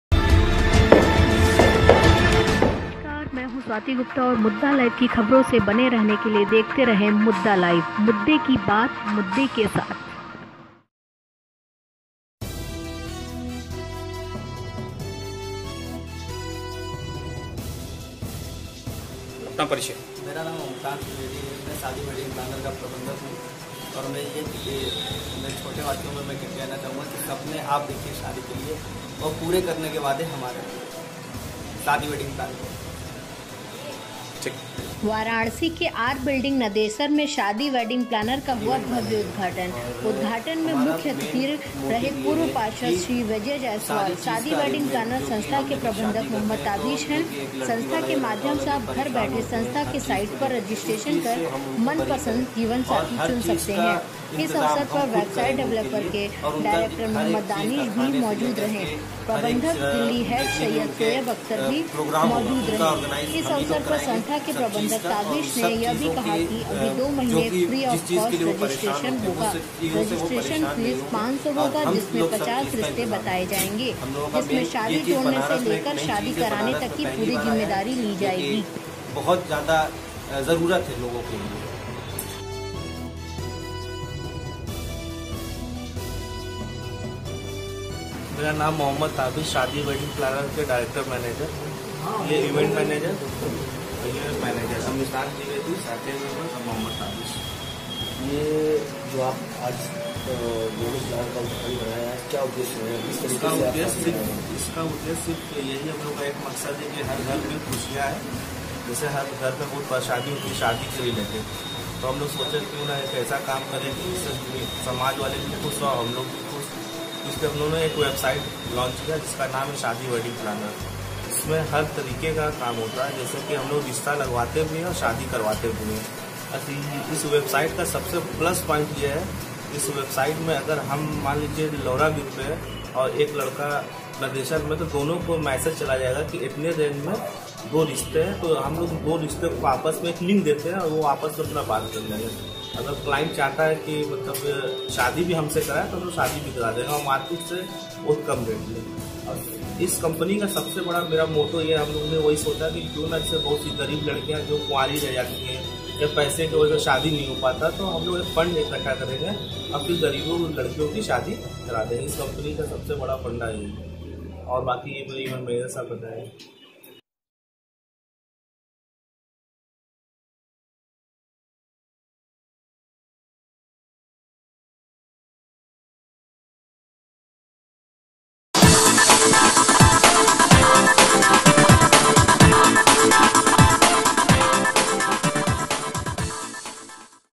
वाराणसी नदेसर स्थित R बिल्डिंग में। शादी वेडिंग प्लानर का हुआ भव्य उद्धघाटन
*वाराणसी के आर बिल्डिंग नदेशर में शादी बेडिंग प्लानर का हुआ भब्य उद्धघाटन*

*उद्द्घाटन में मुख्य अतिथि पूर्व पार्षद श्री विजय जैसवाल थे*

*शादी बेडिंग प्लानर संस्था के प्रबंधक मो0 ताबिश है*

*संस्था के माध्यम से आप घर बैठे संस्था के साईड पर रजिस्ट्रेशन कर मन पसन्द जीवन साथी चुन सकते है* ।

*इस अवसर पर website developer के डायरेक्टर मो0 दानिश भी मौजूद थे*

*प्रबंधक दिल्ही हेड सैयद शोएब अख्तर मौजूद थे*

*इस अवसर पर संस्था के प्रबंधक ताबिश ने कहा अभी दो महीने फ्री आफ कॉस्ट रजिस्ट्रेशन होगा*।

*रजिस्ट्रेशन फीस 500 होगा जिसमें 50 रिस्ते बताए जाएंगे जिसमे शादी जोड़ने से लेकर शादी कराने तक का पूरी जिम्मेदारी किया जाएगा* ।